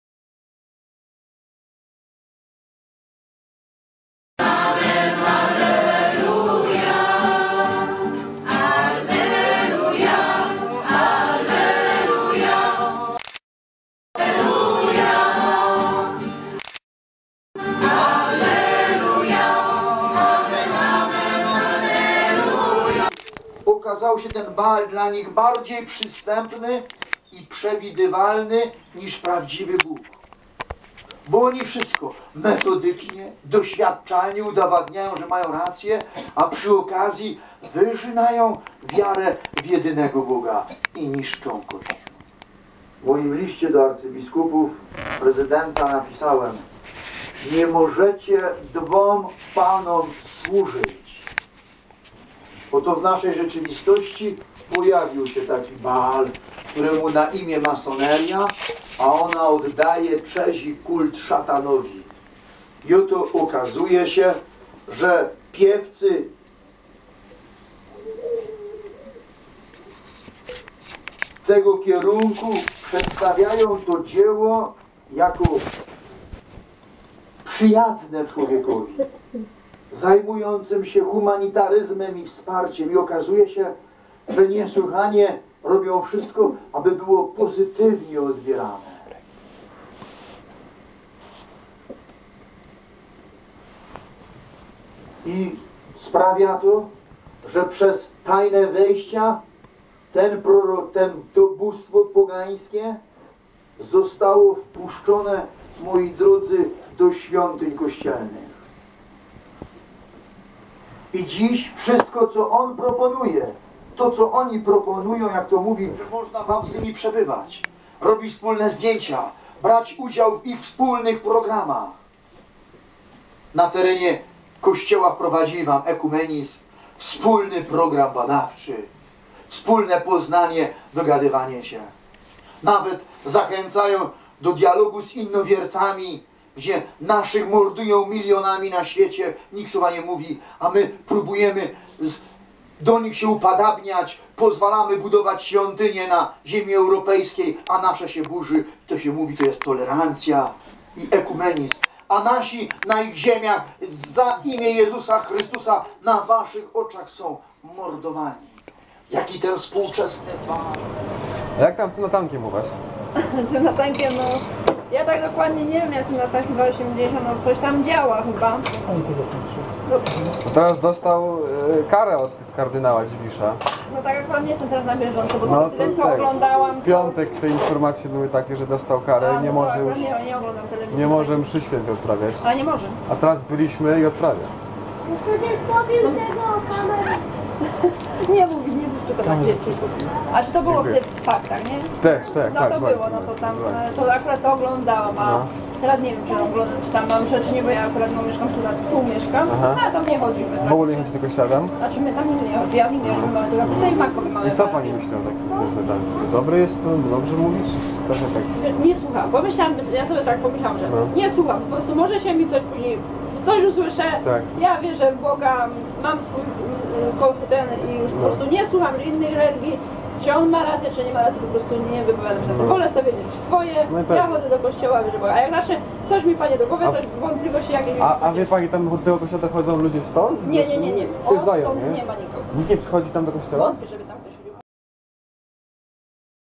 Ksiądz Natanek odprawia nieważną mszę (24.07.2011)
Mimo, że kardynał Stanisław Dziwisz nałożył na księdza Piotra Natanka suspensę, zbuntowany ksiądz odprawił w niedzielę mszę świętą w swojej pustelni.